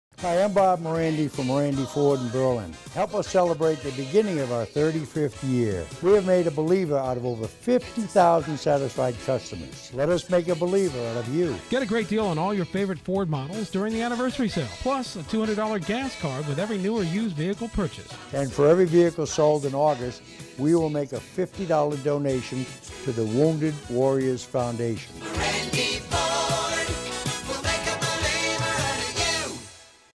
Morande Ford is celebrating their 35th Anniversary!
Morande Ford is celebrating their 35th Anniversary and you're invited to stop by and see all the great deals on your favorite Ford models.